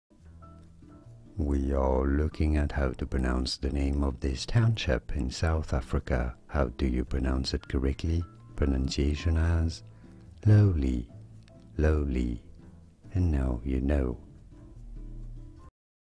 How to Pronounce Lawley?
This video shows you How to Pronounce Lawley, pronunciation guide.

00:00 – How to Pronounce Intro
00:08 - How to Pronounce Lawley
00:20 - More Difficult Pronunciations